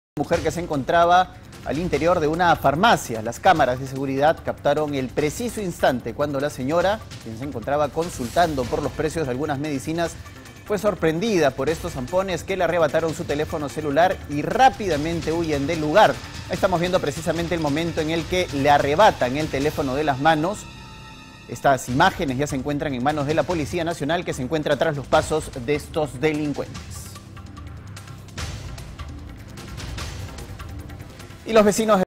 Piura: Delincuentes en mototaxi asaltan a mujer en farmacia
La criminalidad continúa ganando terreno en nuestro país. En Piura, una mujer fue víctima de la inseguridad ciudadana luego que unos delincuentes a bordo de una mototaxi la interceptaran para robarle su celular. La víctima estaba comprando en una farmacia cuando fue asaltada.

Las cámaras de seguridad de dicho establecimiento registraron el incidente. La fémina llegó a la botica en mención para consultar los precios de unas medicinas, cuando fue sorprendida por un ladrón que descendió de una mototaxi. Rápidamente, el hampón le arrebató su celular para huir junto a su cómplice, quien conducía el vehículo.

Con total confusión, impotencia y frustración, la víctima no pudo hacer más que observar cómo los facinerosos escapaban con su dispositivo móvil. Cabe resaltar que los hampones no pudieron robar nada de la farmacia, pues dicho lugar contaba con una reja que protegía a la empleada y los productos.

Policía ya está investigando

Los archivos de las cámaras ya se encuentran en manos de la Policía Nacional, a fin de que puedan realizar las investigaciones correspondientes y capturar a los criminales a la brevedad.